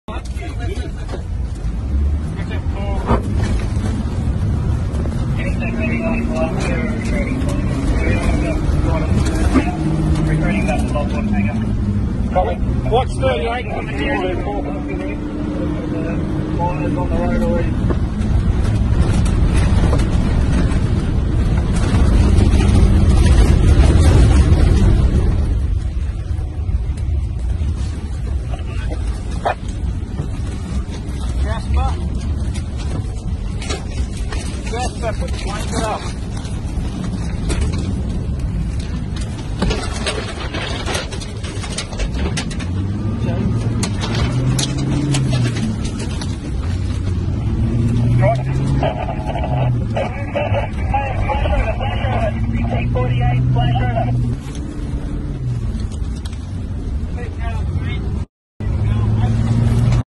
Inside The Heart of a Wildfire
Prayers out for the town of Conjola and for the people all around the world who experienced flames like this.🙏🏼